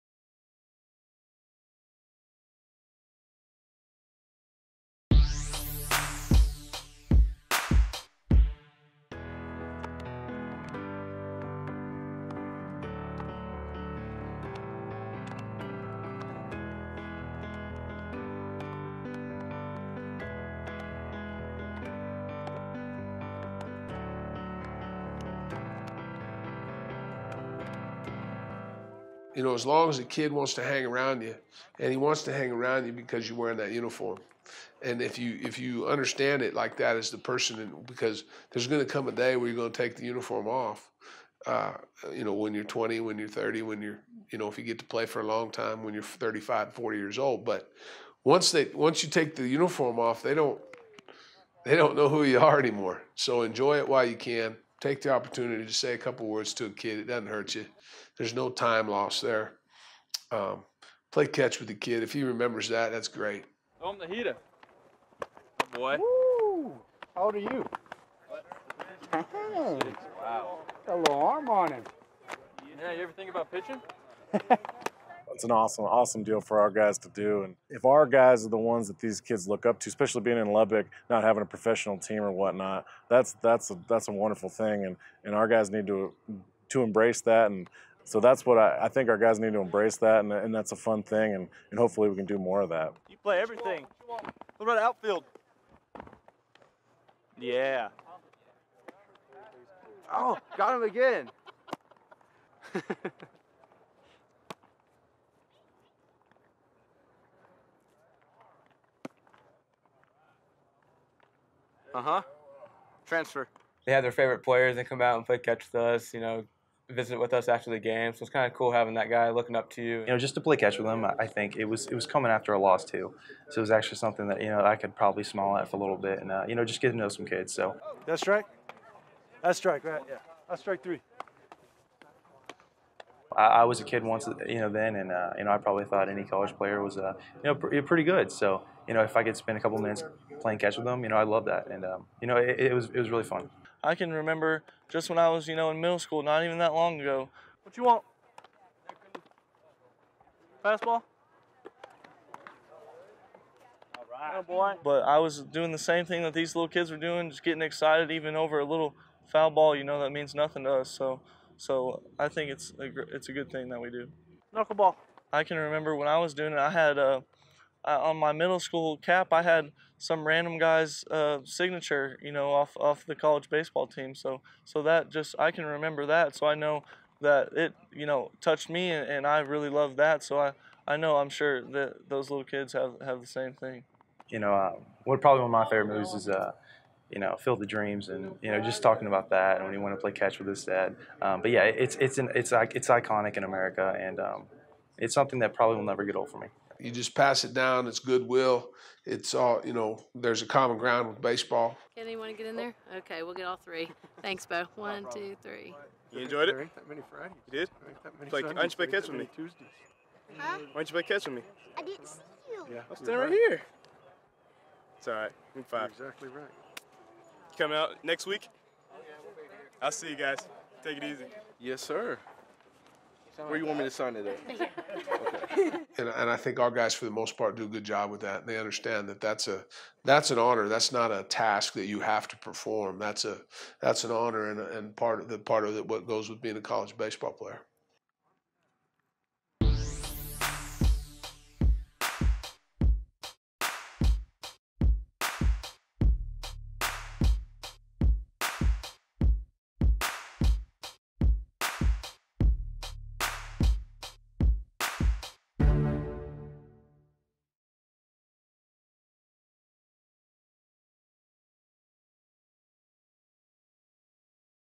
Raider Power: Texas Tech Players Enjoy a Game of Catch with Local Kids
Texas Tech baseball players get the opportunity to do more on the diamond than just play a baseball game. See how spending time with local kids in a game of catch can mean more than the home run hit earlier that day.